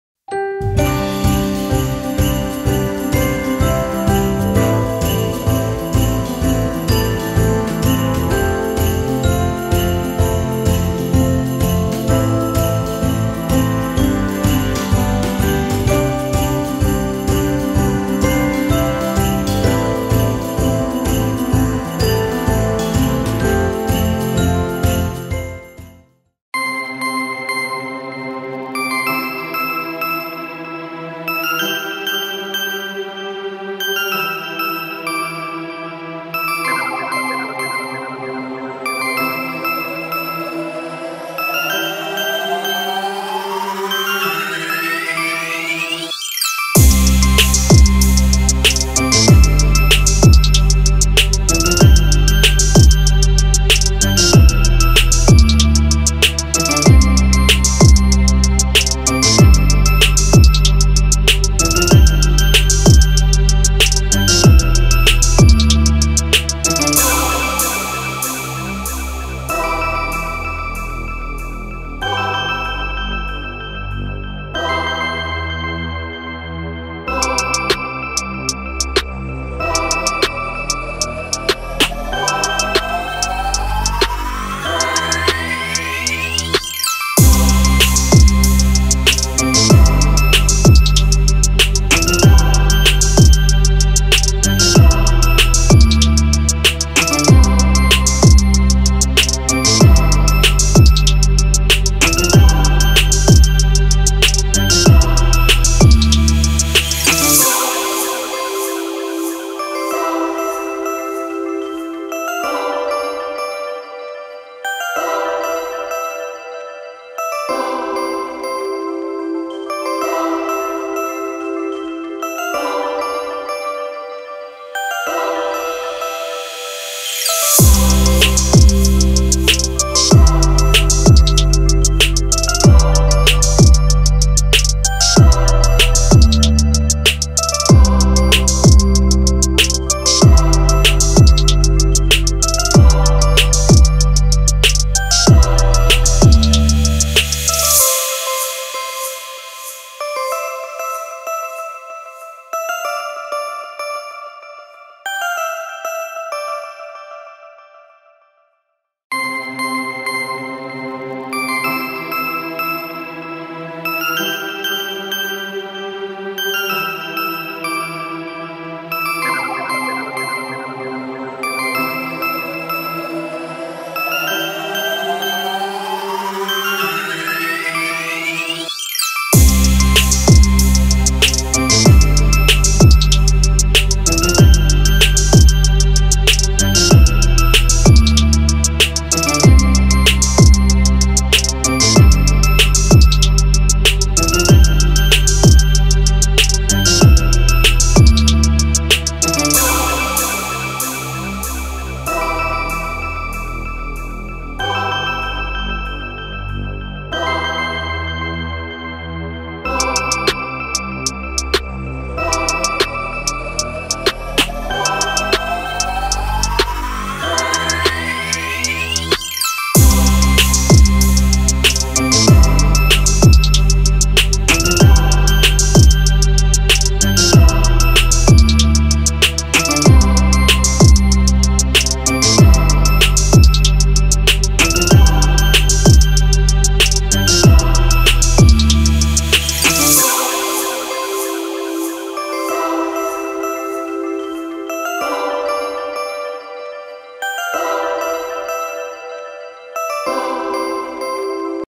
How To Make A Bow With Ribbon | Holiday Basics
Merry Christmas and Happy Holidays! In this video, I will be showing you how to make a simple holiday bow using ribbons!